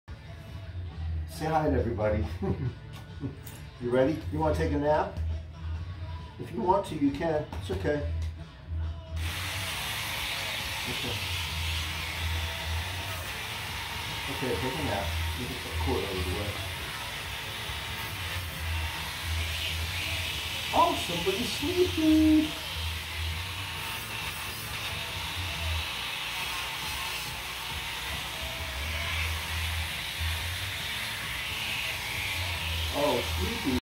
Vizsla puppy yawns and naps through pet-i-cure
Sleepy weepy puppy pedicure